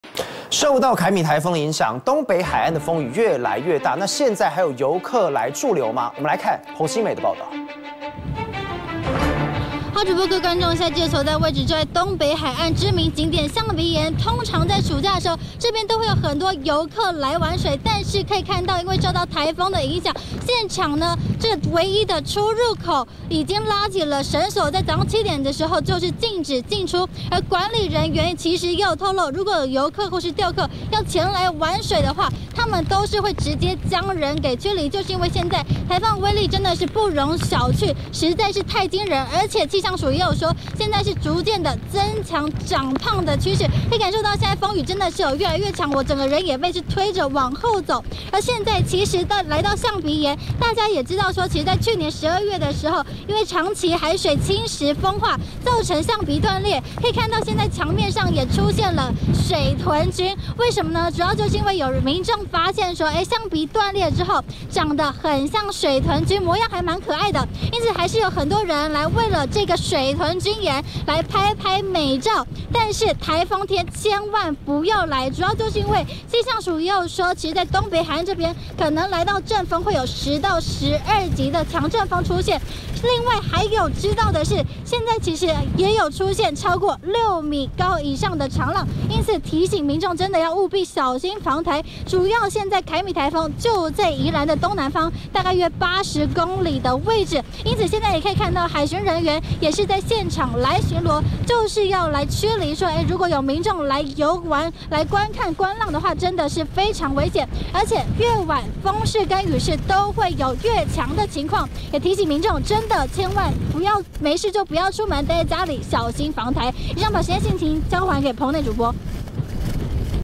【凱米颱風】最新》凱米強襲！東北海岸「象鼻岩」入口 拉繩索管制@newsebc
受到凱米颱風影響，東北海岸風雨越來越大，還有遊客嗎？